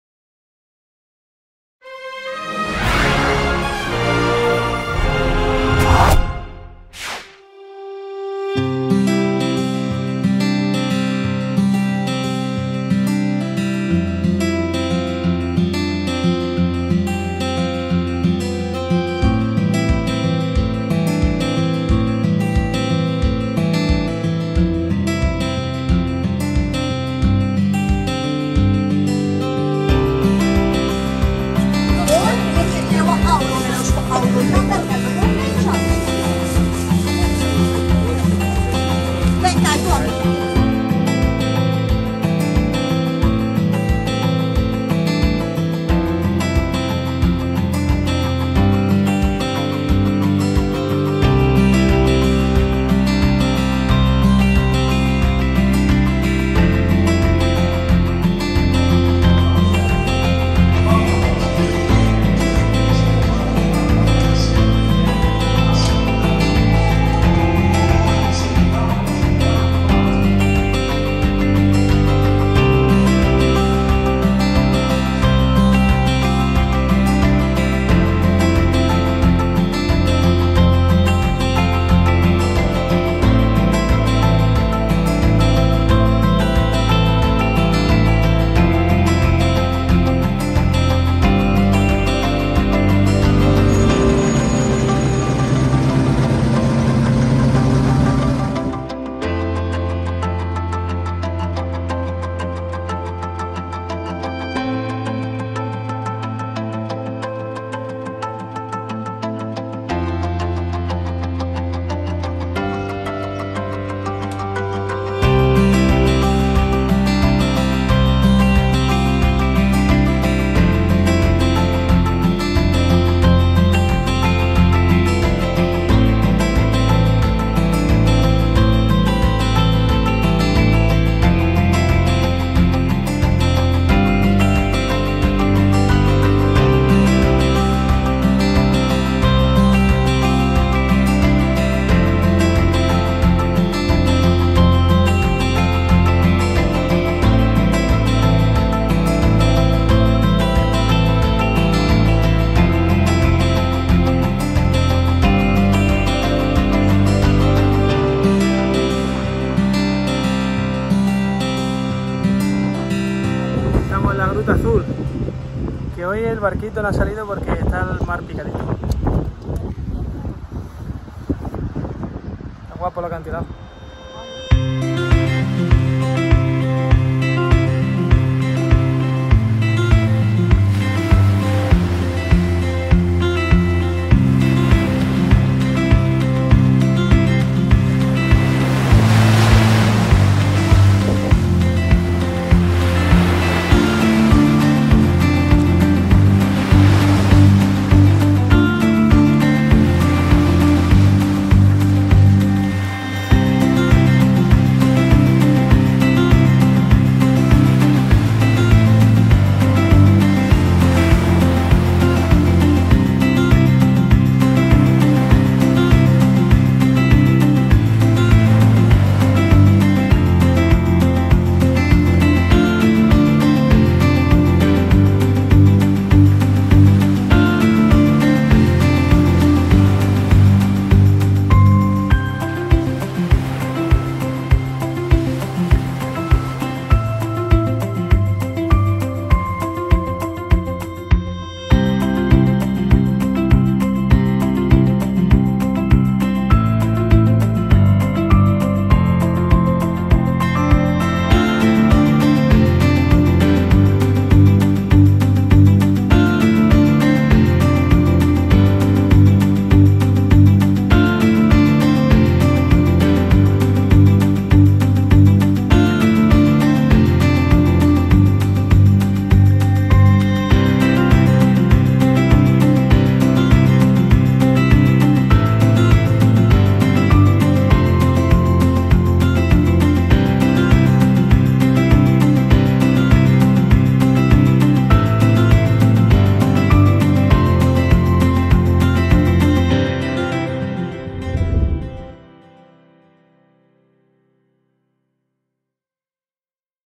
This is Malta 4K. Capítulo 5. Marsaxlokk y Qrendi sin gruta azul
En esta penúltima entrega de Malta conoceremos la localidad de Marsaxlokk, una pequeña villa de gran tradición marinera. Paseando por su puerto veremos sus icónicos luzzus: las tradicionales embarcaciones pesqueras de Malta, caracterizadas por los vivos colores con los que están decoradas, después nos dirigiremos al municipio costero de Wied iz-Zurrieq y aunque no pudimos recorrer su costa en pequeñas embarcaciones  como estaba previsto por motivos climatológicos, si apreciamos desde arriba la gruta azul y esta bonita zona de acantilados con grutas y cuevas marinas  las cuales han sido filmadas en numerosas películas, como por ejemplo el largometraje protagonizado por Brad Pritt Troya. Después y para finalizar visitaremos el pueblo de Qrendi. Marzo 2023